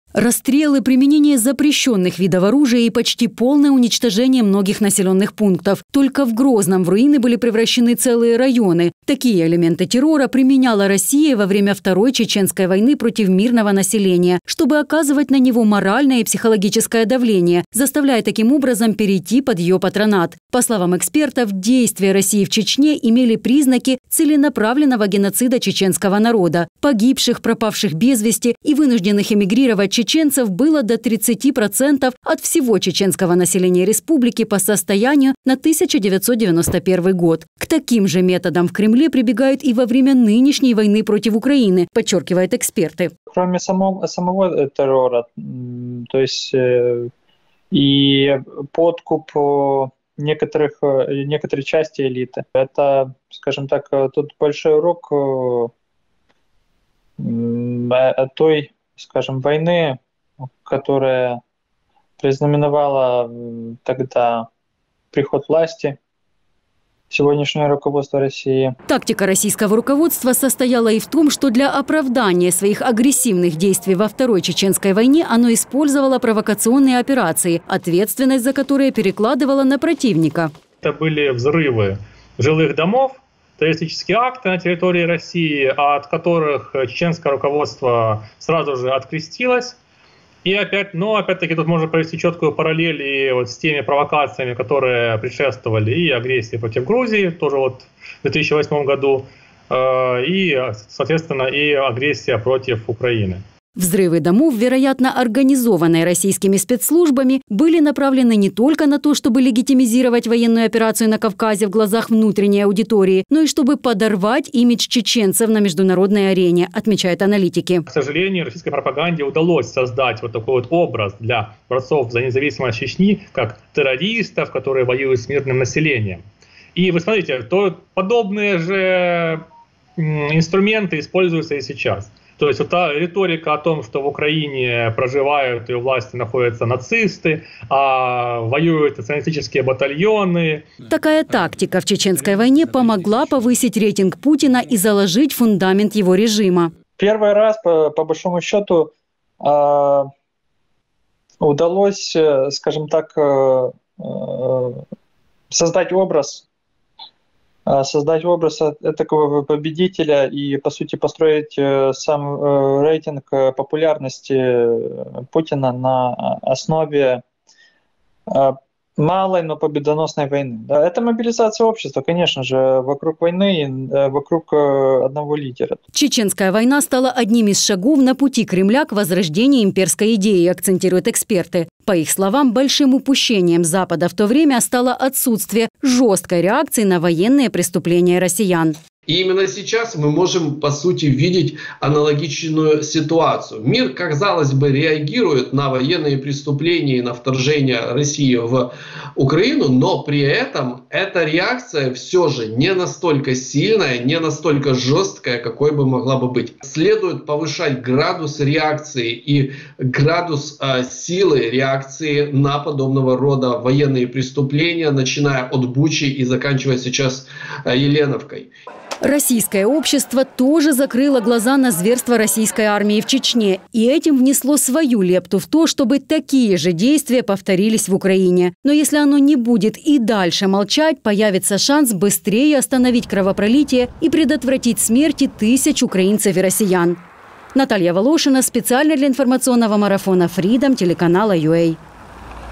Вторая чеченская война. Уроки для Путина
Уже более 12,5 тыс. мирных жителей погибли или получили ранения с момента полномасштабного вторжения России в Украину. Об этом свидетельствуют данные ООН. Это почти столько же жертв, сколько было во Второй чеченской войне, 23-я годовщина с начала которой будет 7 августа. Какие параллели можно провести между этими войнами, смотрите в материале